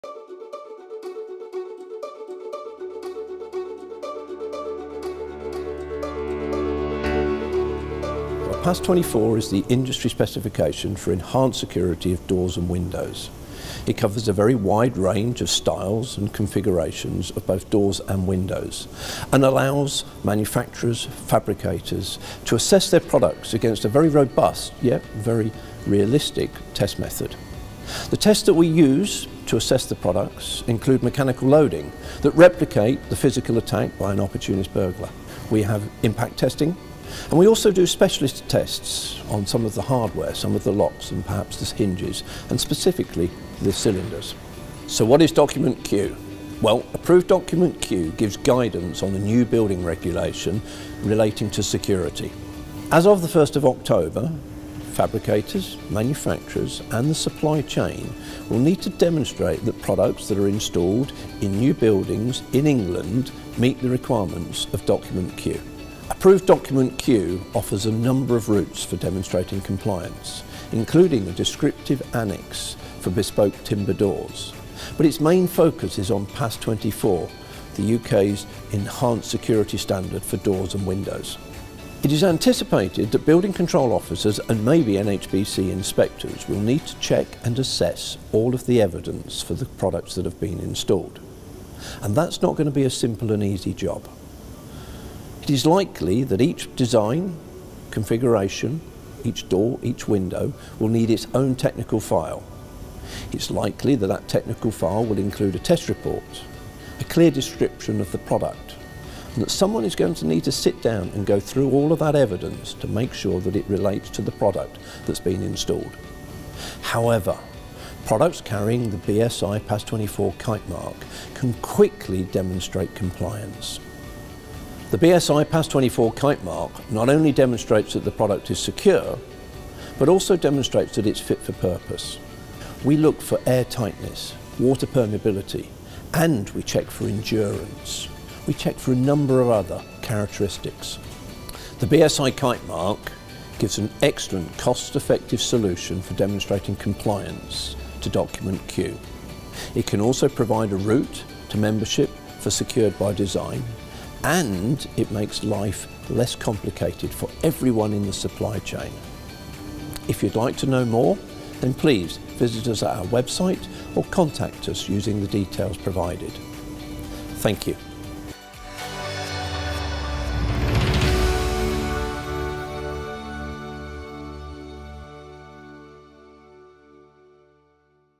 Coping with compliance - PAS 24 & Approved Document Q
Kevin Frewin, BSI's Technical Development Manager, talks about PAS 24, the enhanced security performance requirements for doorsets and windows in the UK and Approved Document Q, a building regulations compliance guide to meet PAS 24 in England.